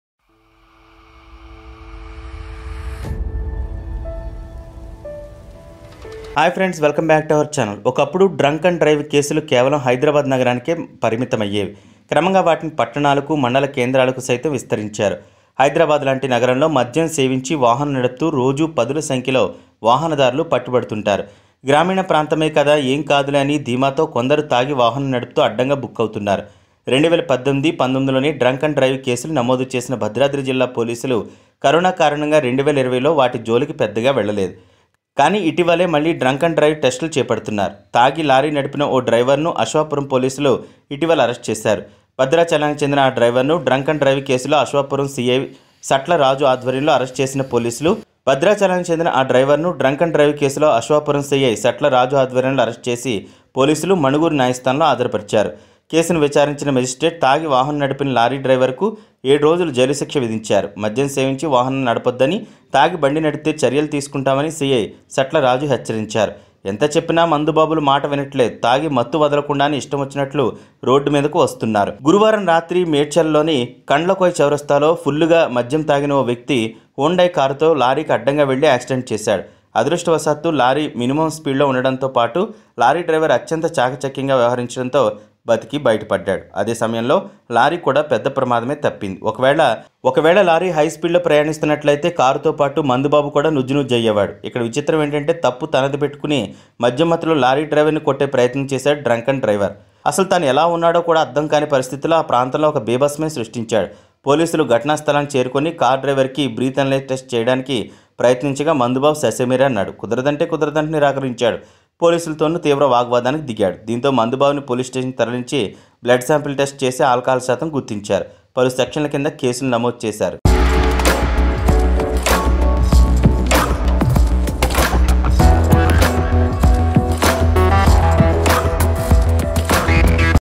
కేవలం 2 నిమిషాల్లో అందరు చూస్తుండగానే కళ్ళముందే ఎంత ఘోరం జరిగిందో మీరే చూడండి || Indian Truck
Watch; కేవలం 2 నిమిషాల్లో అందరు చూస్తుండగానే కళ్ళముందే ఎంత ఘోరం జరిగిందో మీరే చూడండి || Indian Truck